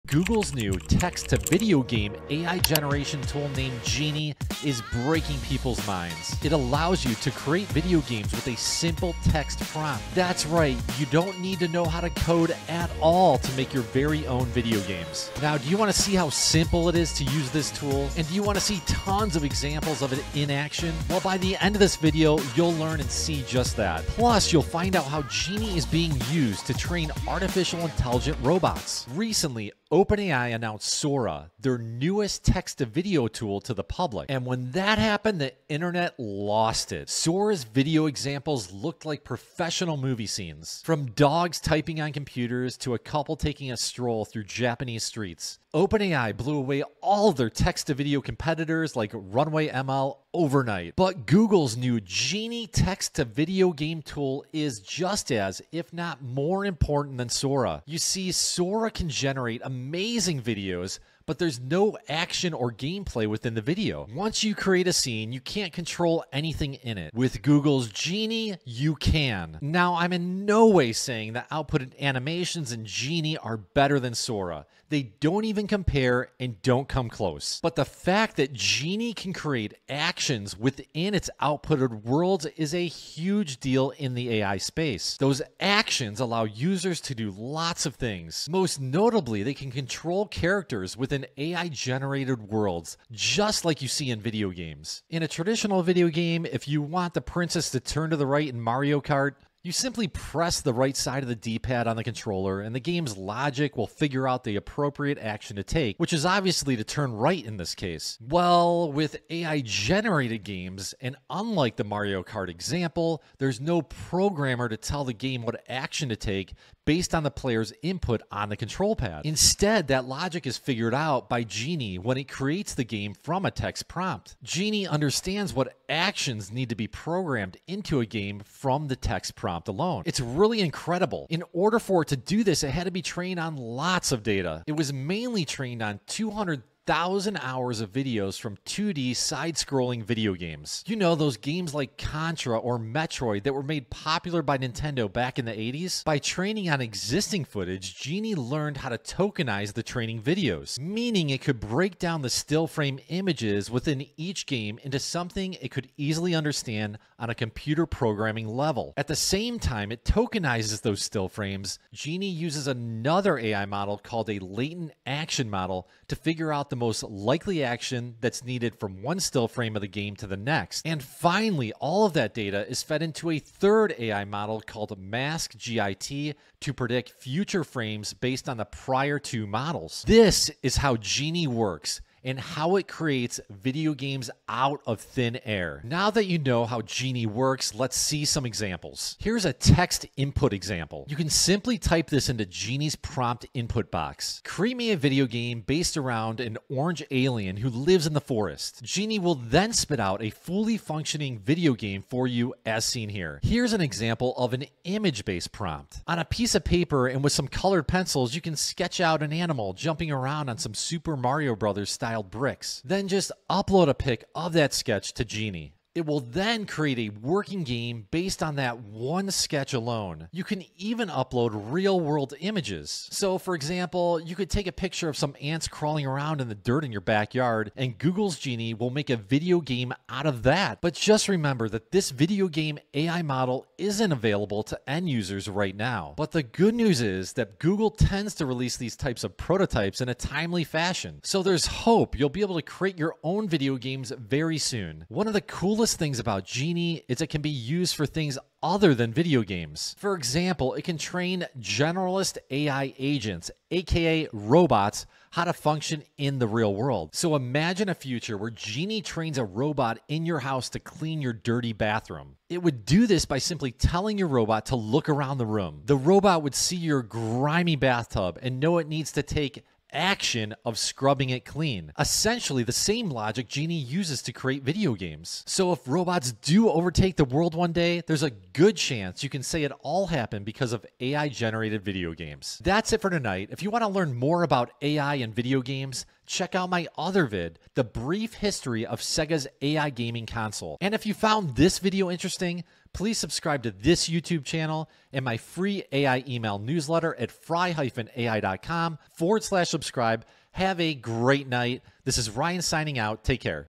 Google Genie: The Text-To-VIDEO GAME AI Generator that is Breaking People’s Minds
Google's new AI tool, Genie, is set to revolutionize the gaming industry by transforming simple text prompts into functioning video games, eliminating the need for coding expertise. The power of Genie goes beyond game development; it can train AI agents, potentially prepping them for real-world tasks. The tool was primarily trained on side-scrolling video games, subsequently understanding the sequence of actions by breaking down the gathered data. This ability to build video games from mere ideas signifies a fascinating shift in game development, democratising access and stirring interest the gaming market.